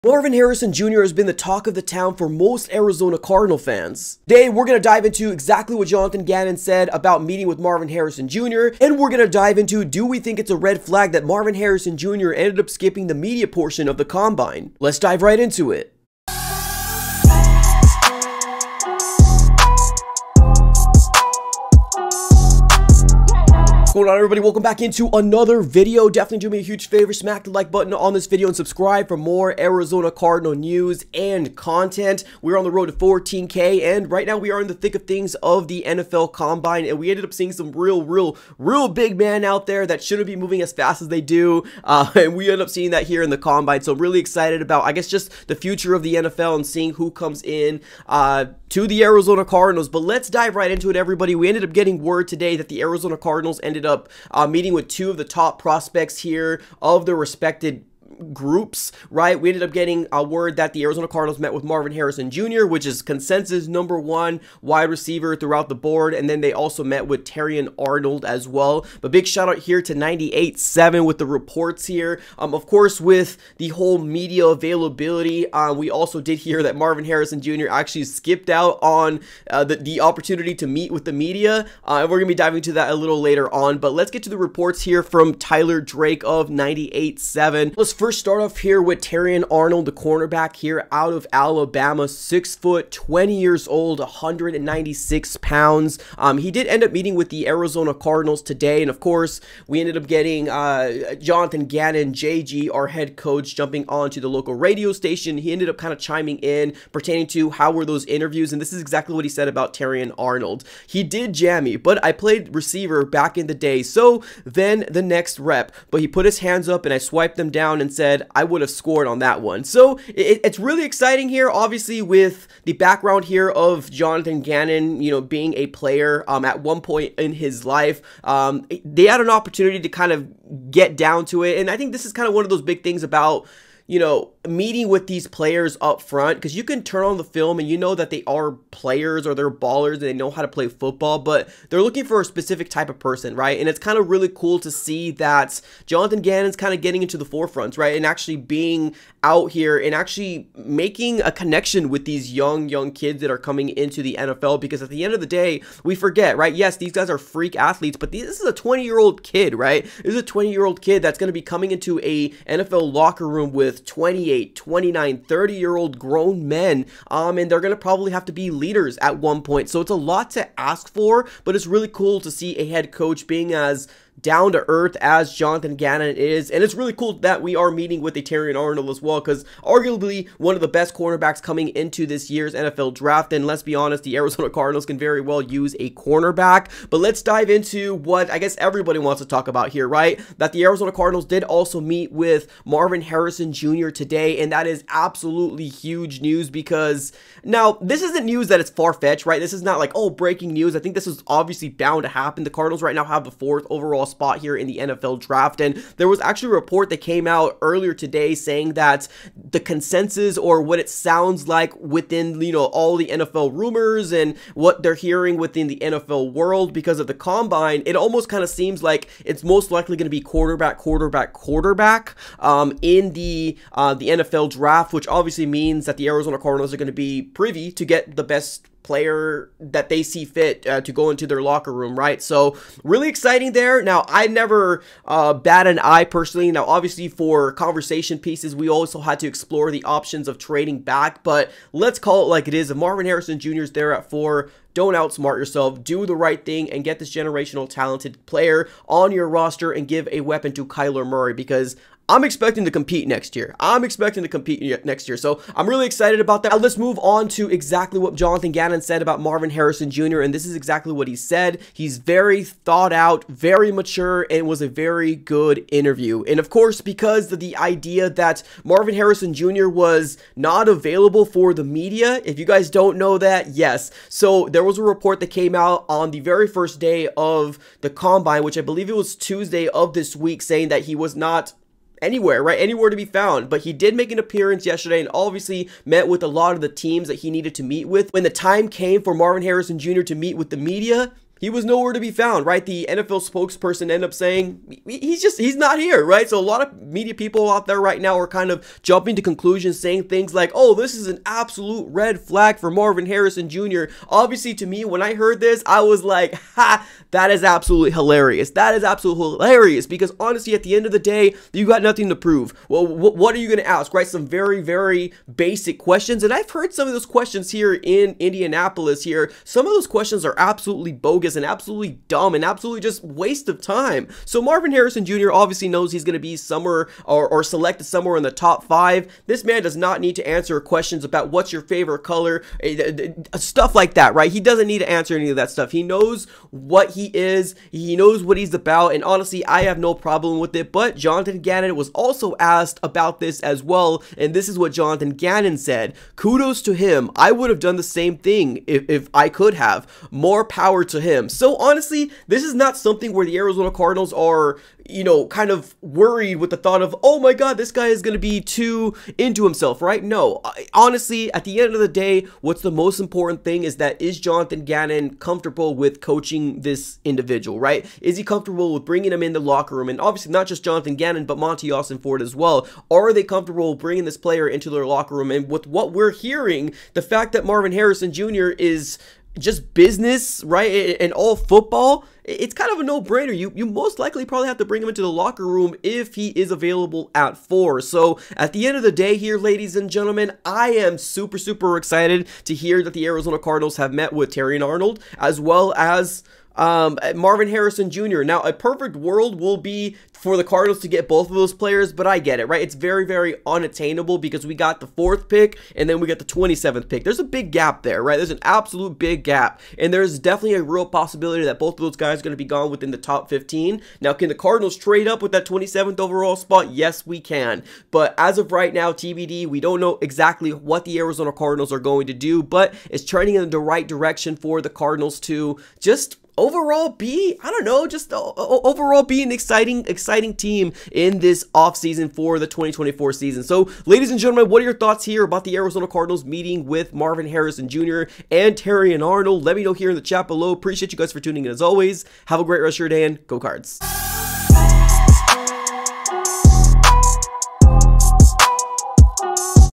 Jonathan Gannon Thoughts On TOP PROSPECTS The Cardinals Met With Today During The NFL Combine!
sold

Chapters:
Intro 00:00 - 00:49
Draft Prospects The Arizona Cardinals Met With Today During The Combine 00:51 - 01:29
Terrion Arnold 01:30 - 03:05
Marvin Harrison Jr. 03:06 - 07:05
In Conclusion 07:06 - 07:30

Today we dive into thoughts from Jonathan Gannon about meeting with Marvin Harrison Jr. and Terrion Arnold.